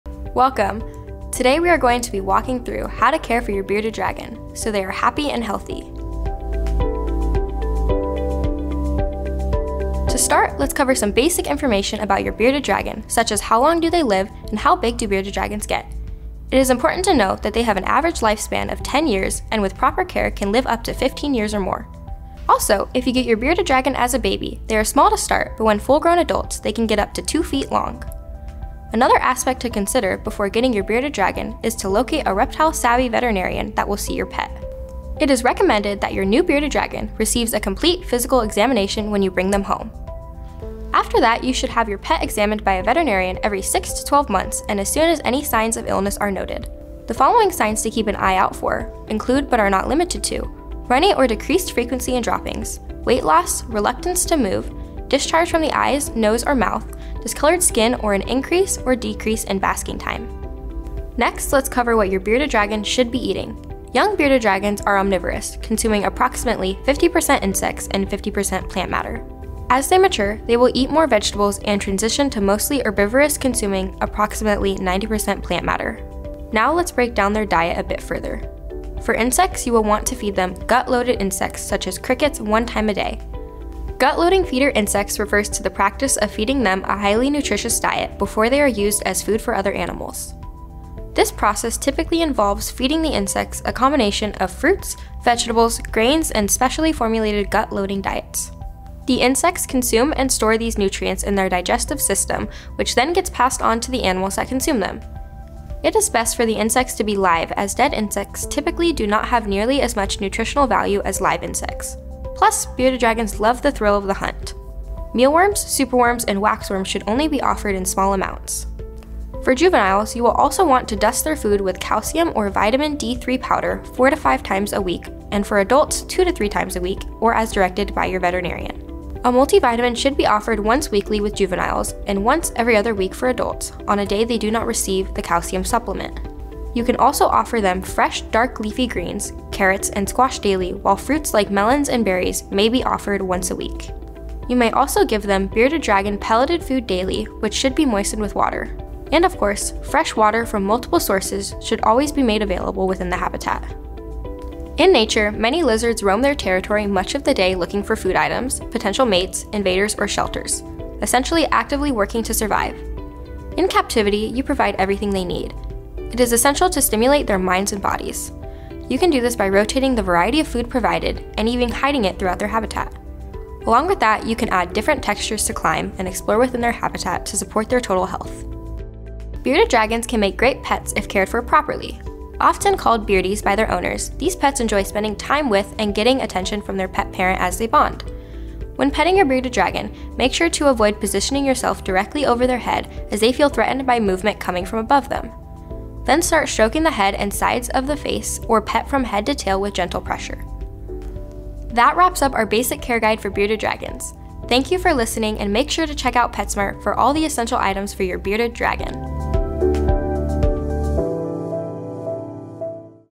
Expert Bearded Dragon Care: Tips for a Thriving Pet
Unlock the secrets to exceptional bearded dragon care. From diet and habitat to lighting and temperature, get all the information you need to ensure your bearded dragon lives a long, happy life.

Get all the supplies you need to take care of your bearded dragon